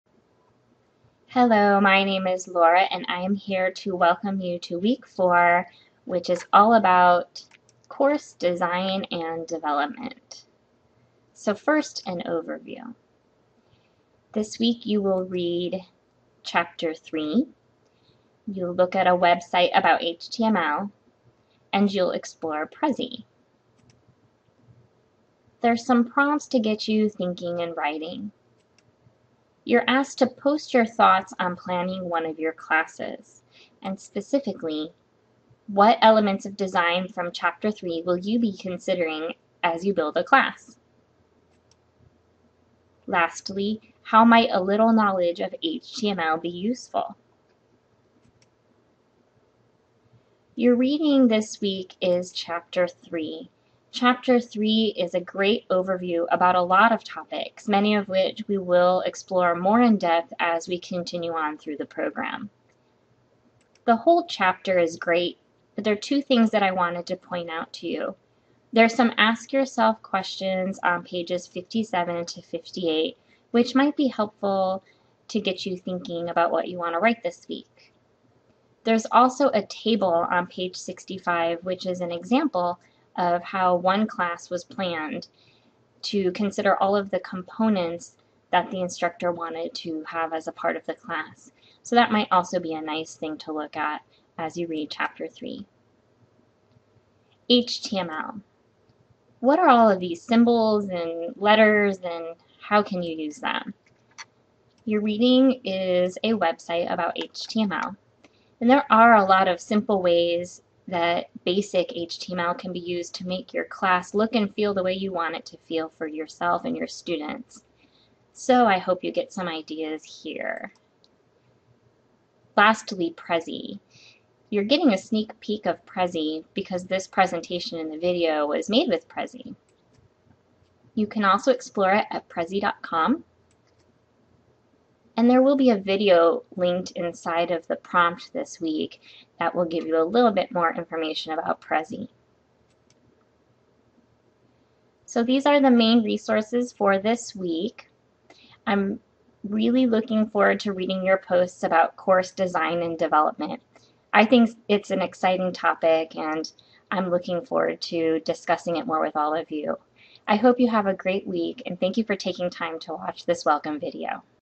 Week 4 Introduction- Program for Online Teaching
This is an introduction video designed to welcome participants to week 4 of the Program for Online Teaching's certificate program at MiraCosta College.  An overview of the week is provided.